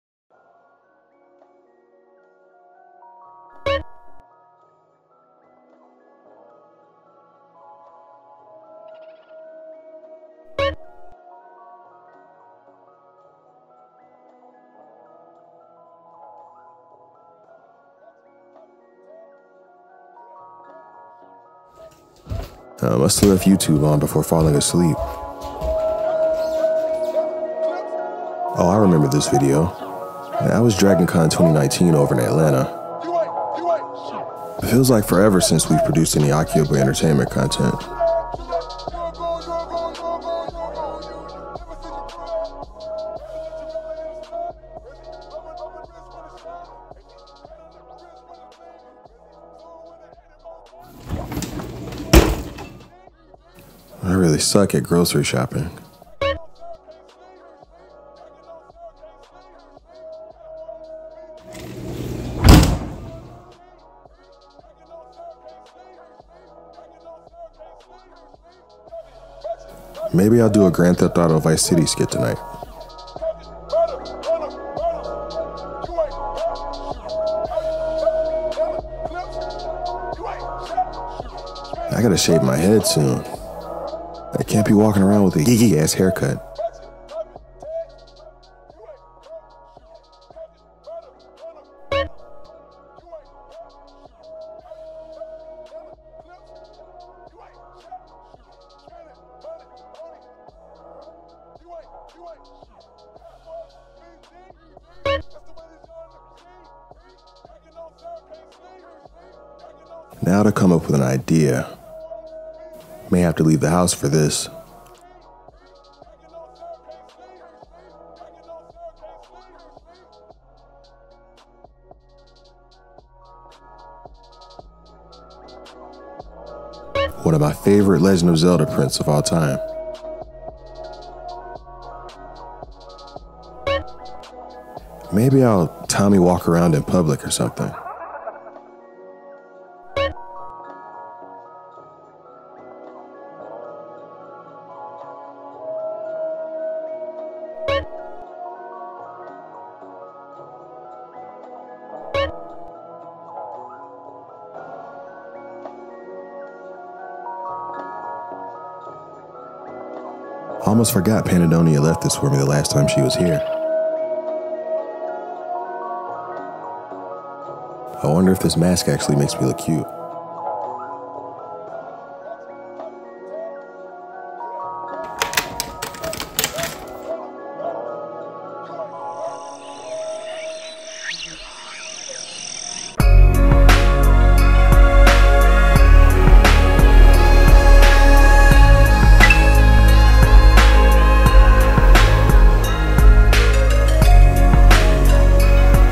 S1: EVERYDAY LIFE AS A VIDEO GAME (PT.2)
Regular situations, but as a video game. 🎮

- + - Cloudie McDoom's Social Links - + -

- + - Cloudie McDoom Store - + -